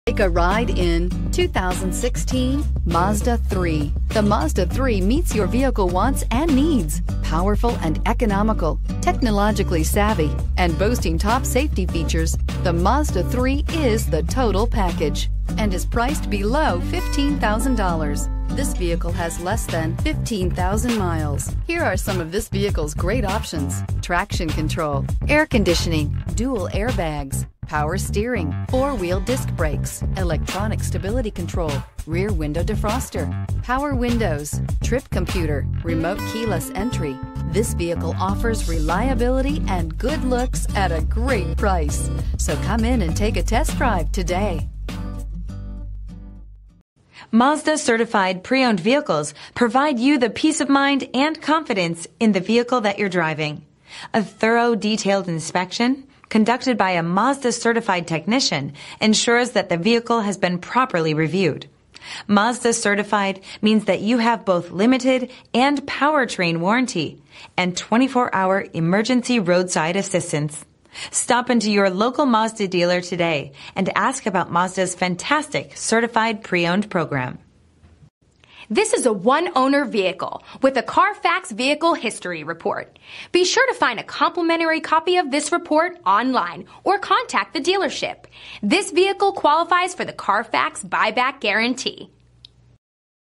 2016 Mazda Mazda3 2016 Mazda Mazda3 i FOR SALE in Las Vegas, NV U1946
CardinaleWay Acura
7000 West Sahara Ave

Certified. CARFAX One-Owner. Clean CARFAX.  Recent Arrival! 2016 Mazda Mazda3 i Sport 30/41 City/Highway MPG  Mazda Certified Pre-Owned Details:      160 Point Inspection     Warranty Deductible:  0     Transferable Warranty     Vehicle History     Includes Autocheck Vehicle History Report with 3 Year Buyback Protection     Powertrain Limited Warranty: 84 Month/100,000 Mile (whichever comes first) from original in-service date     Roadside Assistance     Limited Warranty: 12 Month/12,000 Mile (whichever comes first) after new car warranty expires or from certified purchase date  Awards:     2016 KBB.com 10 Best Sedans Under  25,000  Vehicle price and availability are subject to change without notice.